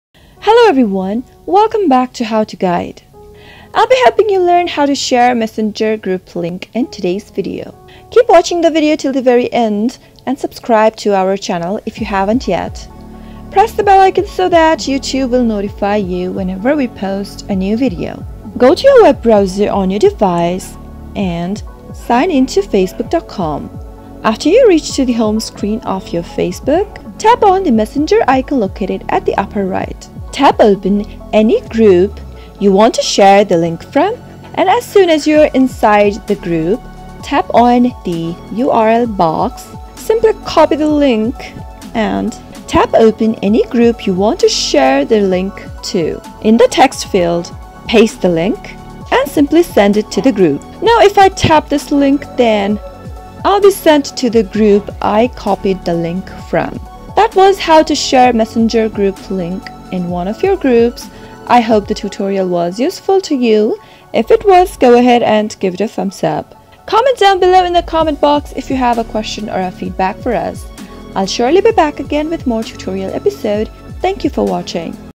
How to Share Messenger Group Link 2021
Share messenger group link iphone | how to share messenger group Link android | how to share messenger group links desktop

This video tutorial walks you through the step by step process on how to share messenger group link with friends in 2021. For this, you need to open your messenger app on your mobile device or go and login to your messenger account. Now, open the group chat you want to share link of. Click on the settings and copy the group link as shown in the video. Now you can share the group link with other users who are already on messenger and they can join the group.